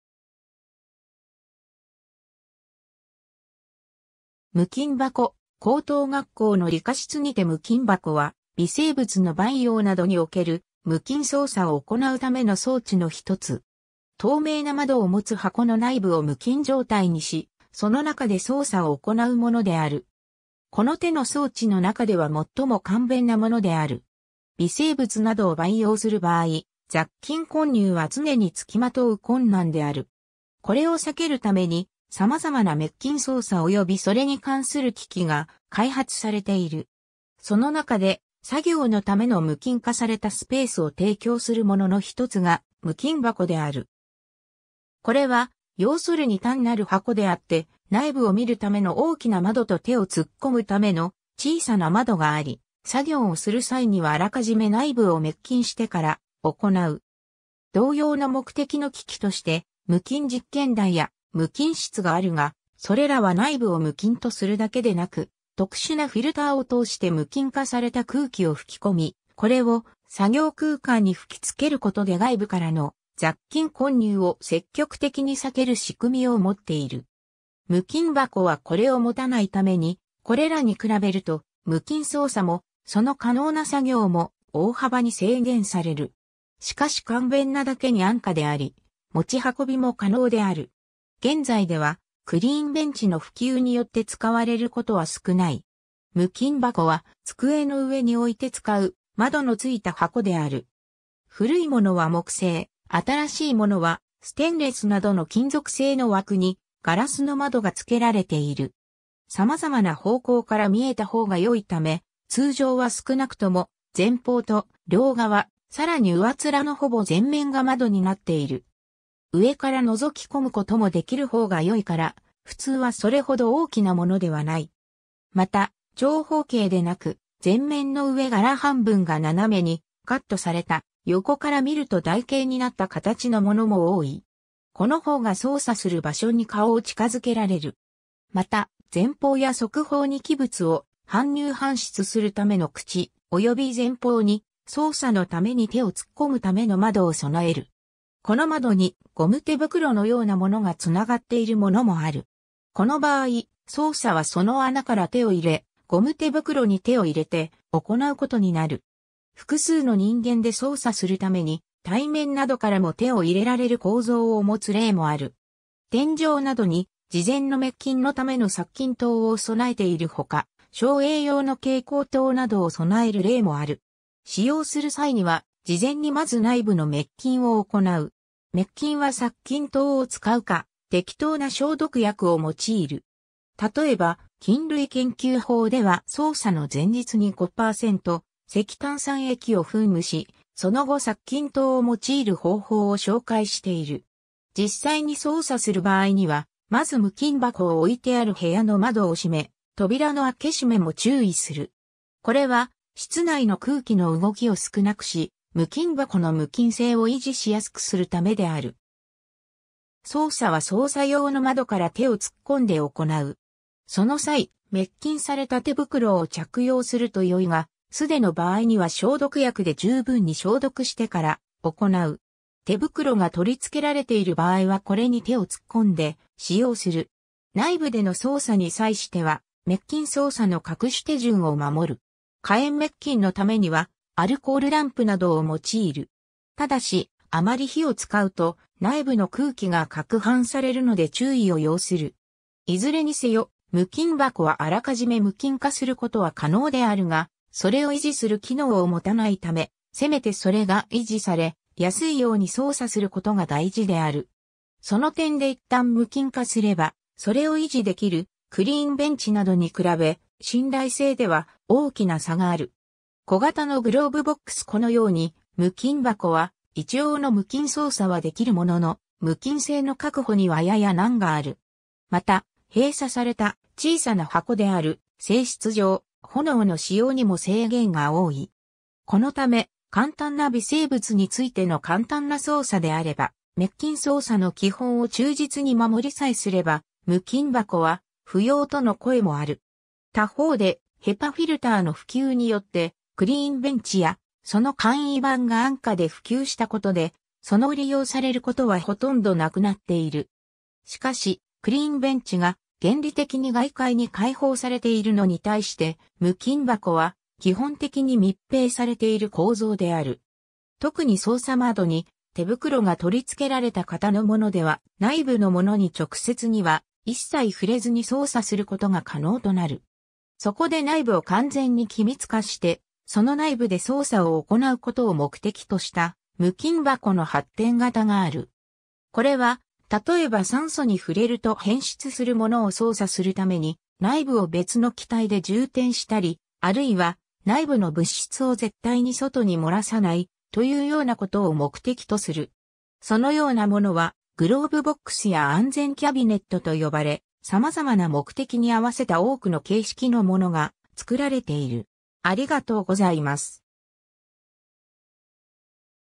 無菌箱
無菌箱・高等学校の理科室にて 無菌箱（むきんばこ）は、微生物の培養などにおける無菌操作を行うための装置の1つ。
透明な窓を持つ箱の内部を無菌状態にし、その中で操作を行うものである。
この手の装置の中では最も簡便なものである。
 微生物などを培養する場合、雑菌混入は常につきまとう困難である。
これを避けるために様々な滅菌操作およびそれに関する機器が開発されている。
その中で、作業のための無菌化されたスペースを提供するものの1つが無菌箱(clean box)である。
これは、要するに単なる箱であって、内部を見るための大きな窓と手を突っ込むための小さな窓があり、作業をする際にはあらかじめ内部を滅菌してから行う。
 同様の目的の機器として無菌実験台（クリーンベンチ）や無菌室があるが、それらは内部を無菌とするだけでなく、特殊なフィルターを通して無菌化された空気を吹き込み、これを作業空間に吹き付けることで外部からの雑菌混入を積極的に避ける仕組みを持っている。
無菌箱はこれを持たないために、これらに比べると無菌操作もその可能な作業も大幅に制限される。
しかし簡便なだけに安価であり、持ち運びも可能である。
現在ではクリーンベンチの普及によって使われることは少ない。
 無菌箱は、机の上に置いて使う窓のついた箱である。
古い物は木製、新しい物はステンレスなどの金属製の枠にガラスの窓がつけられている。
様々な方向から見えた方が良いため、通常は少なくとも前方と両側、さらに上面のほぼ全面が窓になっている。
上からのぞき込むこともできる方がよいから、普通はそれほど大きなものではない。
また、長方形でなく、前面の上柄半分が斜めにカットされた、横から見ると台形になった形のものも多い。
この方が操作する場所に顔を近づけられる。
 また、前方や側方に器物を搬入搬出するための口、および前方に操作のために手を突っ込むための窓を備える。
この窓にゴム手袋のようなものが繋がっているものもある。
この場合、操作はその穴から手を入れ、ゴム手袋に手を入れて行うことになる。
複数の人間で操作するために、対面などからも手を入れられる構造を持つ例もある。
 天井などに事前の滅菌のための殺菌灯を備えている他、照影用の蛍光灯などを備える例もある。
 使用する際には、事前にまず内部の滅菌を行う。
滅菌は殺菌灯を使うか、適当な消毒薬を用いる。
例えば『菌類研究法』では操作の前日に5%石炭酸液を噴霧し、その後殺菌灯を用いる方法を紹介している。
 実際に操作する場合には、まず無菌箱を置いてある部屋の窓を閉め、扉の開け閉めも注意する。
これは室内の空気の動きを少なくし、無菌箱の無菌性を維持しやすくするためである。
操作は操作用の窓から手を突っ込んで行う。
その際、滅菌された手袋を着用するとよいが、素手の場合には消毒薬で十分に消毒してから行う。
手袋が取り付けられている場合はこれに手を突っ込んで使用する。
 内部での操作に際しては、滅菌操作の各種手順を守る。
火炎滅菌のためにはアルコールランプなどを用いる。
ただし、あまり火を使うと内部の空気が攪拌されるので注意を要する。
 いずれにせよ、無菌箱はあらかじめ無菌化することは可能であるが、それを維持する機能を持たないため、せめてそれが維持されやすいように操作することが大事である。
その点でいったん無菌化すればそれを維持できるクリーンベンチなどに比べ、信頼性では大きな差がある。
 小型のグローブボックス このように、無菌箱は一応の無菌操作はできるものの、無菌性の確保にはやや難がある。
また、閉鎖された小さな箱である性質上、炎の使用にも制限が多い。
このため、簡単な微生物についての簡単な操作であれば、滅菌操作の基本を忠実に守りさえすれば無菌箱は不要との声もある。
他方でHEPAフィルターの普及によって、クリーンベンチやその簡易版が安価で普及したことで、その利用されることはほとんどなくなっている。
 しかし、クリーンベンチが原理的に外界に解放されているのに対して、無菌箱は基本的に密閉されている構造である。
特に操作窓に手袋が取り付けられた型のものでは、内部のものに直接には一切触れずに操作することが可能となる。
そこで内部を完全に気密化して、その内部で操作を行うことを目的とした無菌箱の発展型がある。
これは、例えば酸素に触れると変質するものを操作するために内部を別の気体で充填したり、あるいは内部の物質を絶対に外に漏らさない、というようなことを目的とする。
そのようなものはグローブボックスや安全...